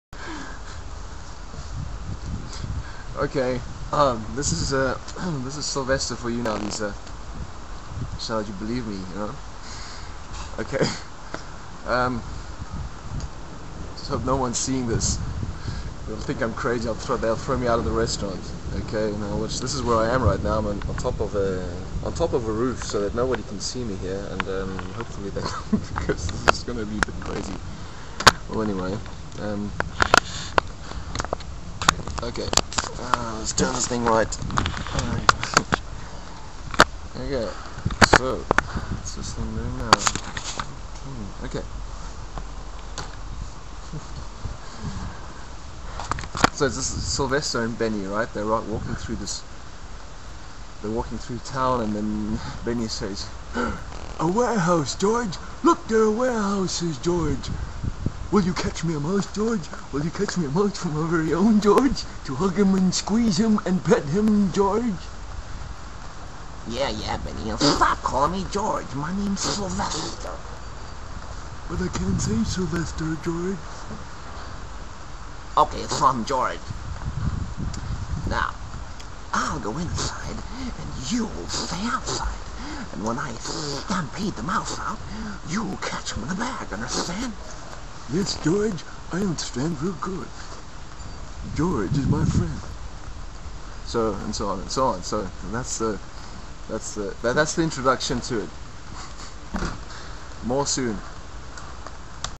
Silvester 1
Hey, I actually did this one for Lisa cause she did'nt want to believe its me over the phone. So I had to prove it to her on video ;) .. I sent her the file privately at first. When I first saw this, I flooped myself :)) .. - Lisa had her laugh on it too, so we opted to put this thing on the Tube :)) ... I used to do this in front of the class at school at times and it cracked everyone up .. we had fun man, we were the best class ever ! .. well, its been a while .. yet still going .. :)) ..